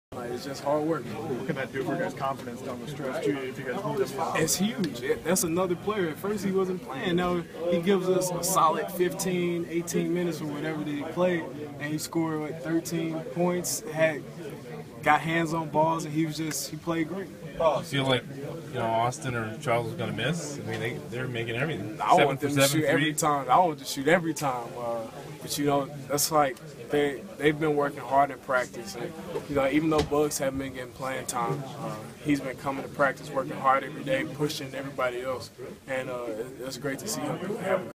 Gophers guard Andre Hollins talks about if freshman Charles Buggs can continue to contribute moving
Gophers guard Andre Hollins talks about if freshman Charles Buggs can continue to contribute moving forward.